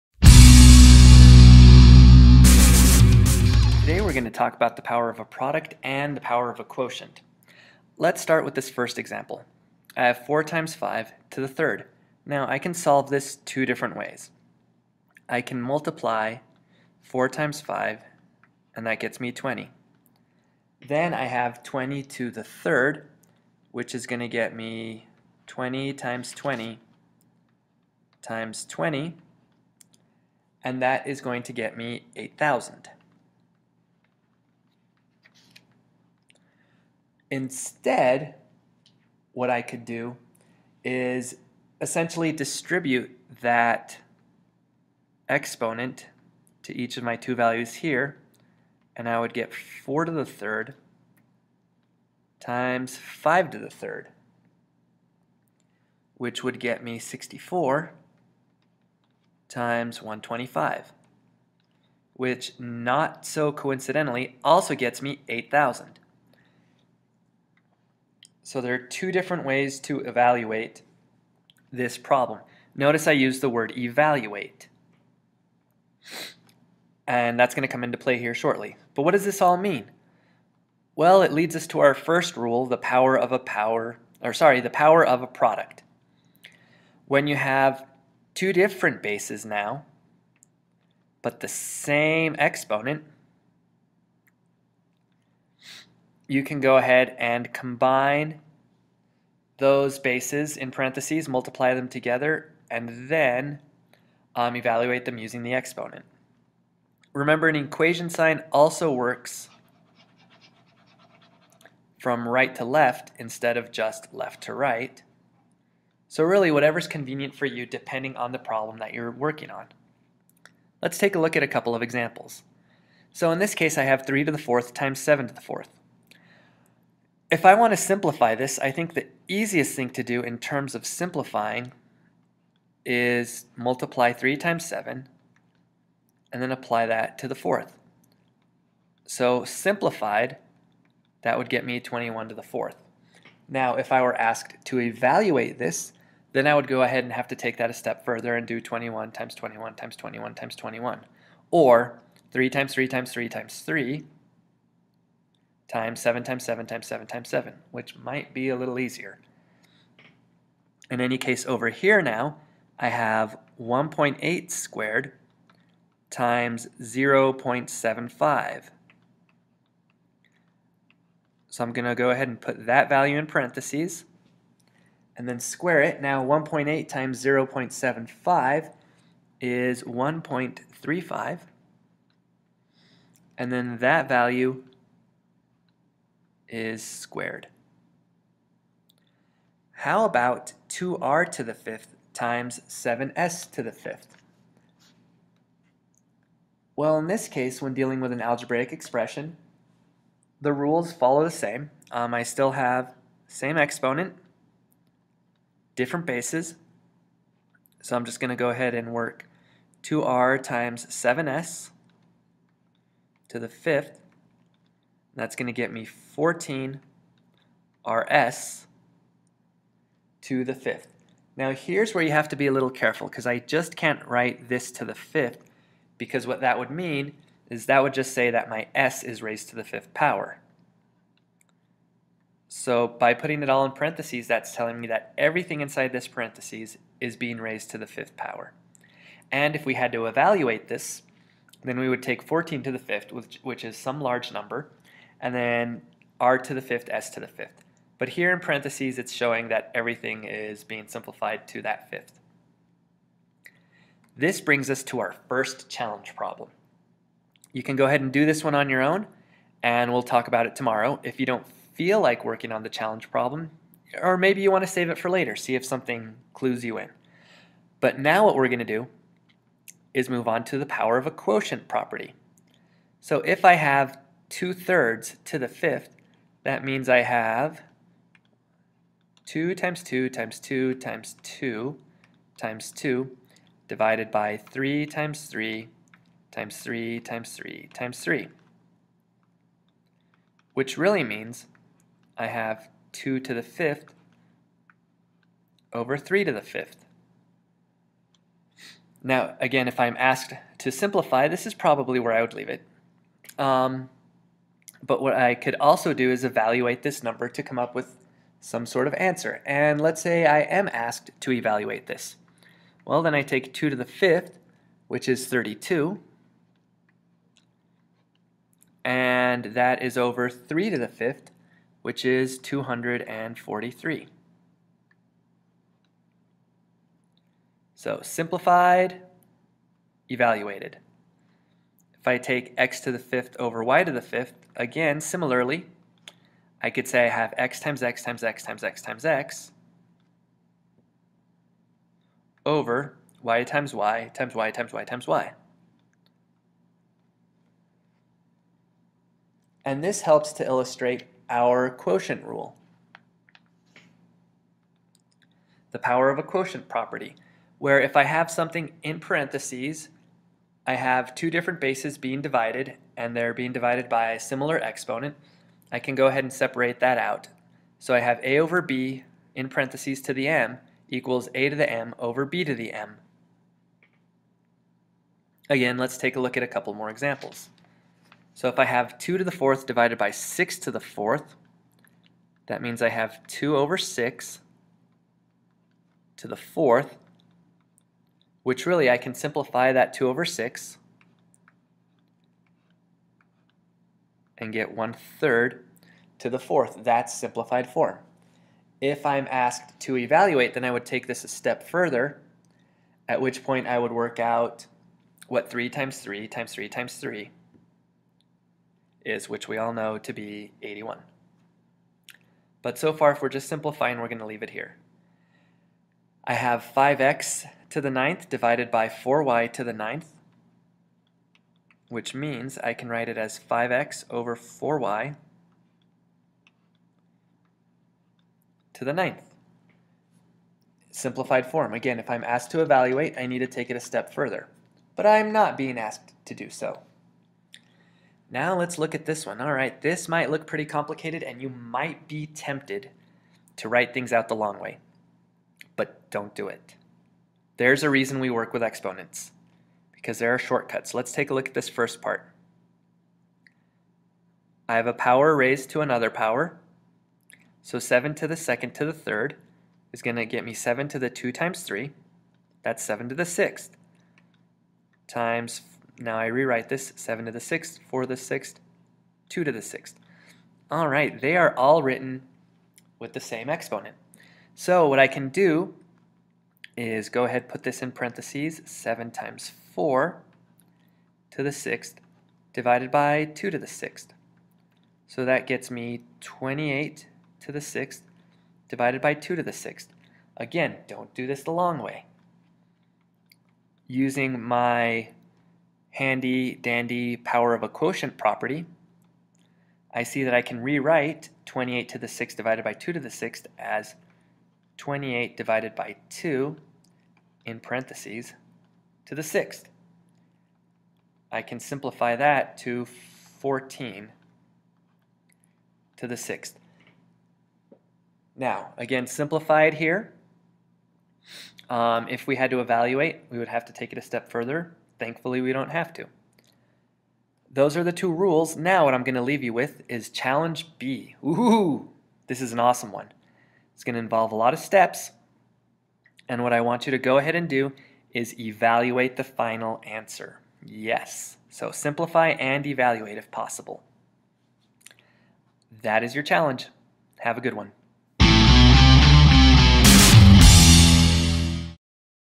1.4 The Power of a Product and the Power of a Quotient
In this video we take a look at the power of a product and the power of a quotient properties, and we use them to simplify expressions.